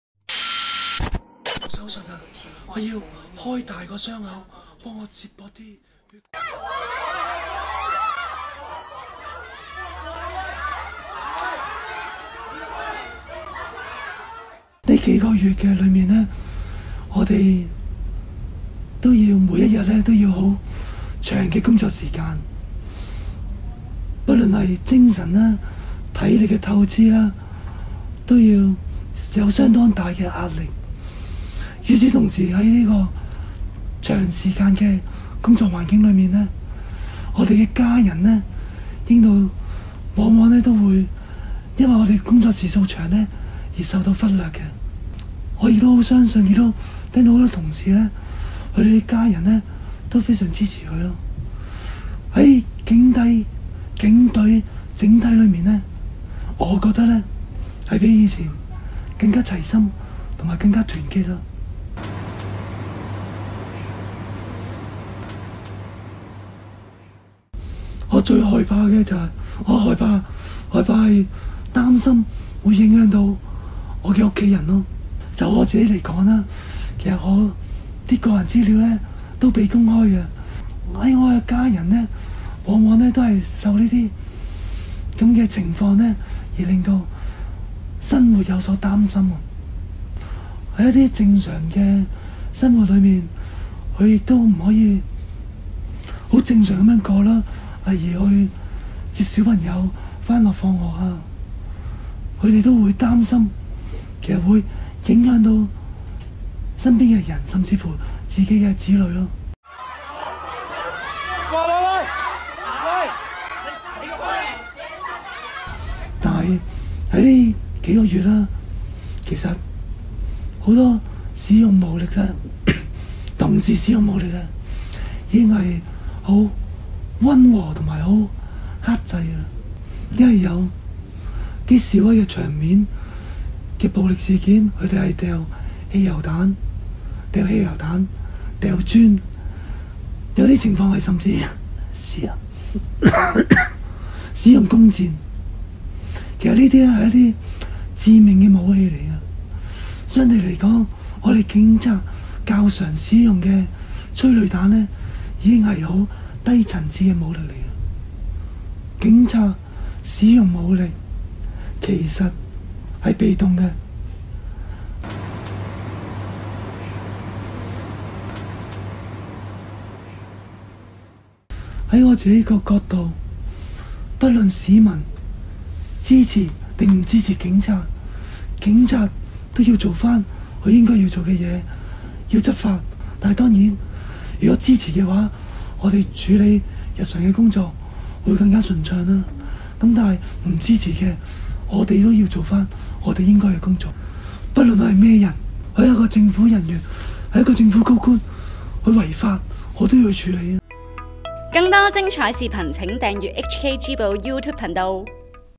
割頸警細訴執法者辛酸：工時長 被起底 影響家人 但警隊更齊心更團結！加油！撐你！
警察在反修例風暴中嘅辛酸，你知道幾多？遭暴徒割頸的勇警Alex內地傳媒《新京報》訪問，用他那虛弱的聲線慢慢說出遇襲後的體會。他表示，近期警察工時好長，往往忽略家人，而且隨時連累家人被起底埋一份，日常生活大受影響。不過，他慶幸警眷都好支持一眾警員，而面對種種困難，警隊比以往更齊心、更團結添！

不只Alex，所有執法者都加油！撐你！

原圖、原片：我們視頻片段